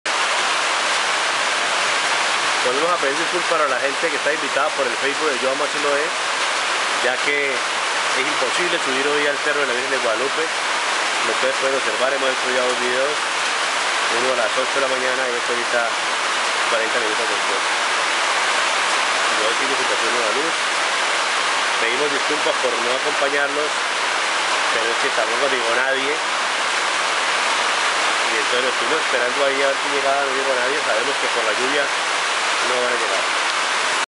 CON CINCO CALEÑOS SUBIMOS AL CERRO DE LA VIRGEN DE GUADALUPE
A PESAR DE LA LLUVIA SUBIMOS CON LOAS UNICAS PERSONAS QUE LLEGARON AL PUNTO  DE ENCUENTRO.
HUBO MUCHO BARRO Y NOS MOJAMOS ,PERO AL MENOS CUMPLIMOS CON LOS QUE DESAFIARON LA LLUVIA.